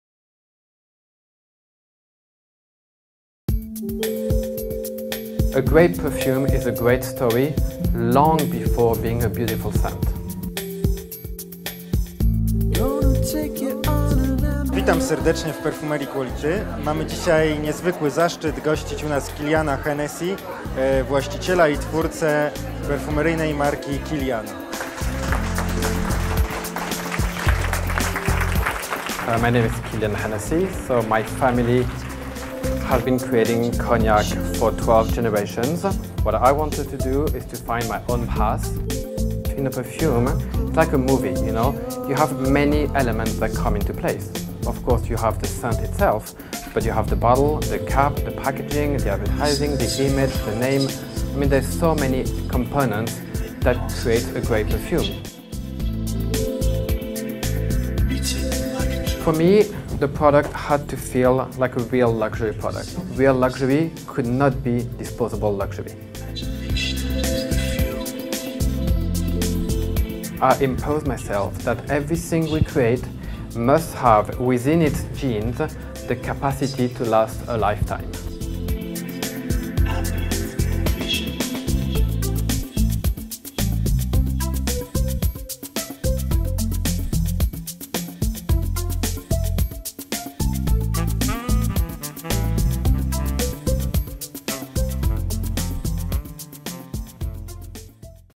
Kilian Hennessy w Perfumerii Quality Missala [napisy PL], 10 lipca 2014
Kilian Hennessy, twórca i właściciel marki By Kilian, z wizytą w Perfumerii Quality Missala, 10 lipca 2014